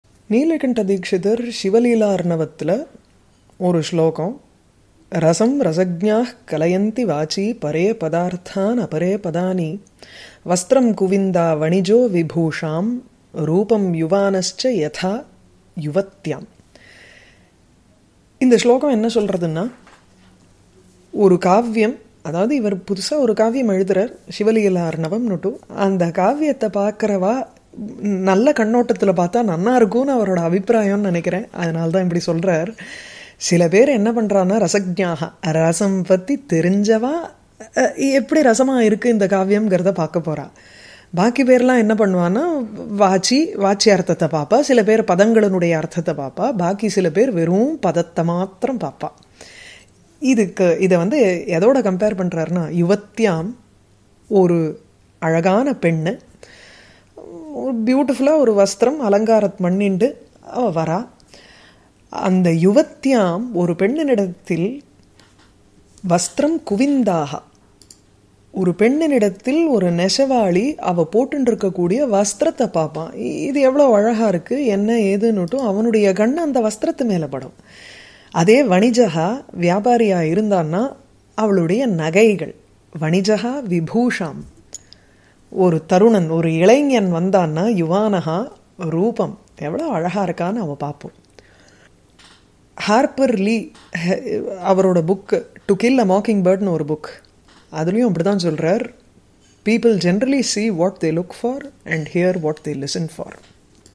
Beholder's eye
वक्त्री → श. विशालाक्षी

रसं रसज्ञाः कलयन्ति वाचि परे पदार्थानपरे पदानि ।
वस्त्रं कुविन्दा वणिजो विभूषां रूपं युवानश्च यथा युवत्याम् ॥

A person of refined taste enjoys poetry for it's 'rasa', others look at word meanings and yet others appreciate mere words. Likewise, in a woman, the weaver looks at her clothes, a merchant her jewels and a youth her form.